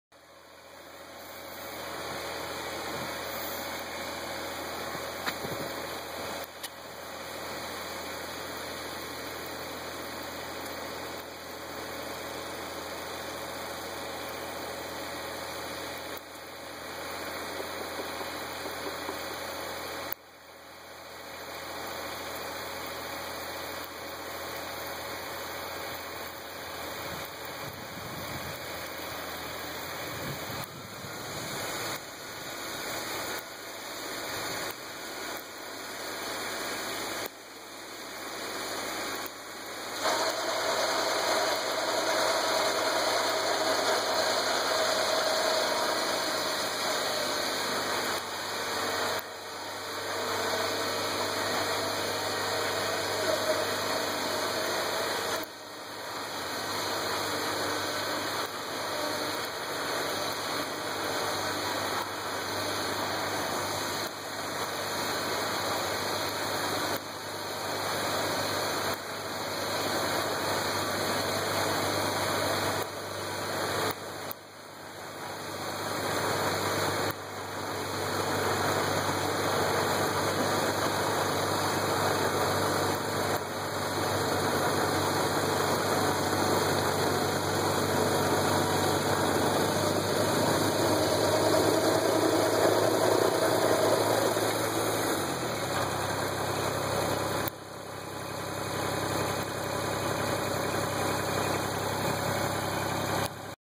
Kubota BX2750D casting distance
Demonstration of the casting distance of the Kubota BX2750D snow thrower using a BX24 tractor.